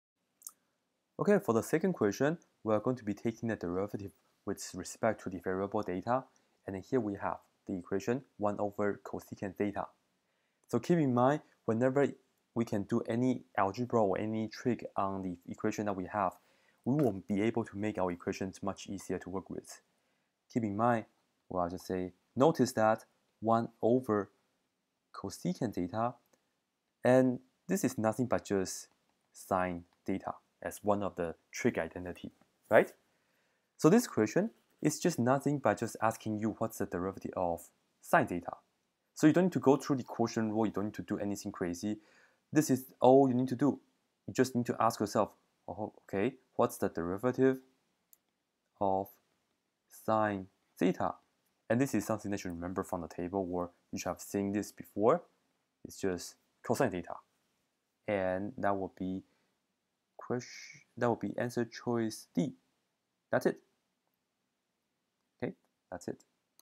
(Q2.) So, you think you can take the derivative?
Take the derivative of 1/csc(x)